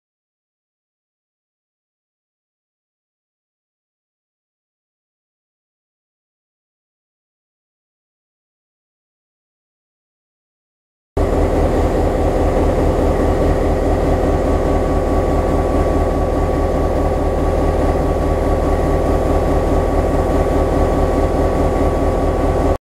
Contecrail - MY 1158 diesel engine power sound at night
Filmed at Vojens Station.